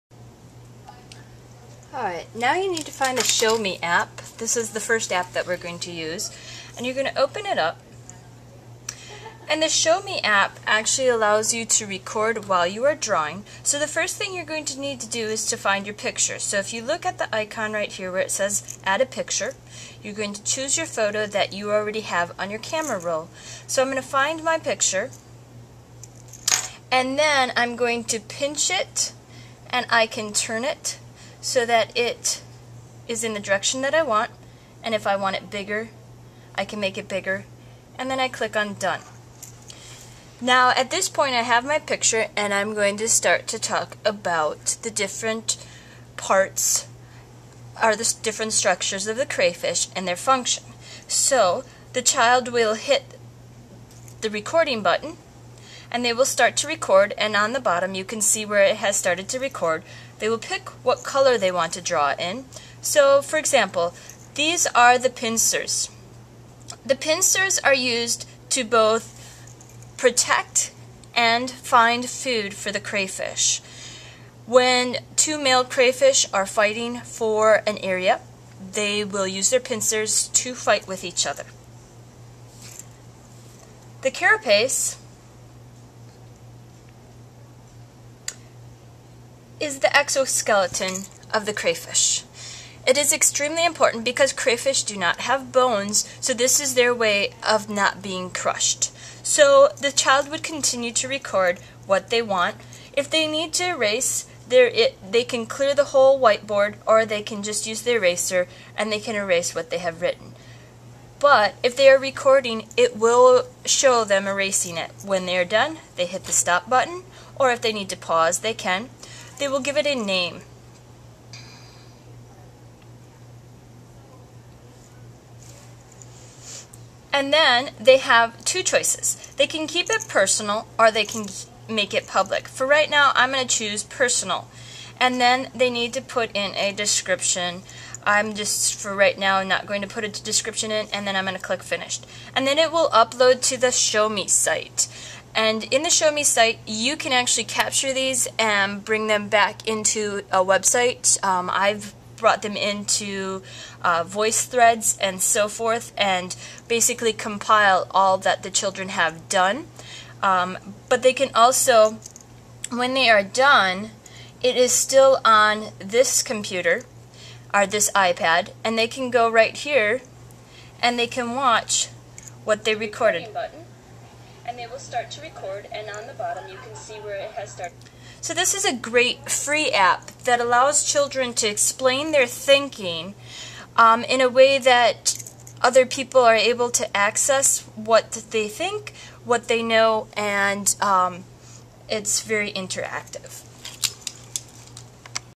ShowMe App Introduction by Karie Huttner
Learn how to use images in the ShowMe App.